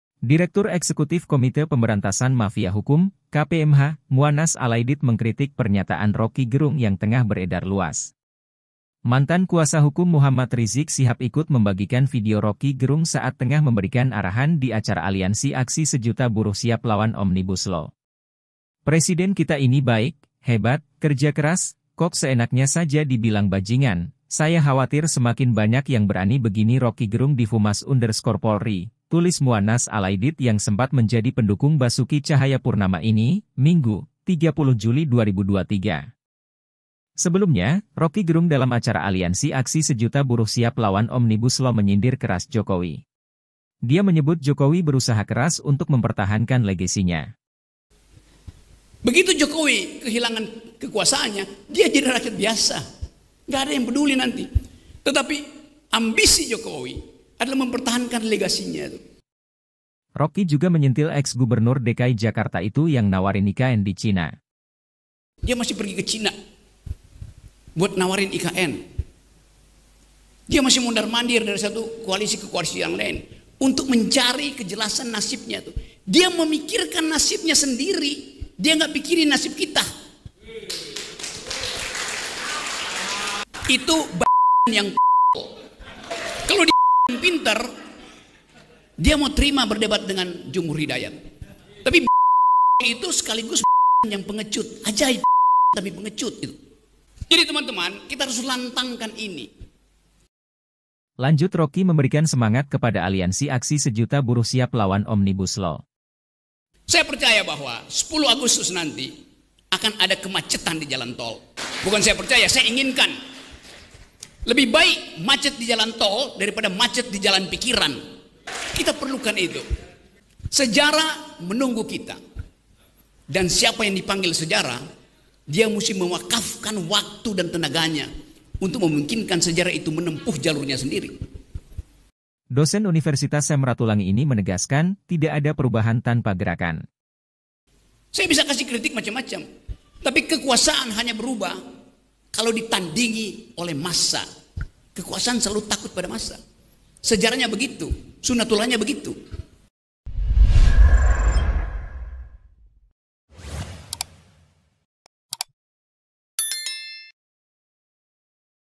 Rocky Gerung Sebut Jokowi Baj*ngan, Muannas Alaidid: Presiden Kita Ini Baik, Hebat, Kerja Keras
Direktur eksekutif Komite Pemberantasan Mafia Hukum (KPMH), Muannas Alaidid mengkritik pernyataan Rocky Gerung yang tengah beredar luas.

Mantan Kuasa Hukum Muhammad Rizieq Shihab ikut membagikan video Rocky Gerung saat tengah memberikan arahan di acara Aliansi Aksi Sejuta Buruh Siap Lawan Omnibus Law.

“Presiden kita ini baik, hebat, kerja keras, kok seenaknya saja di bilang bajingan, saya khawatir semakin banyak yang berani begini @rockygerung @DivHumas_Polri,” tulis Muannas Alaidid yang sempat menjadi pendukung Basuki Tjahaja Purnama (Ahok) ini, Minggu, (30/7/2023).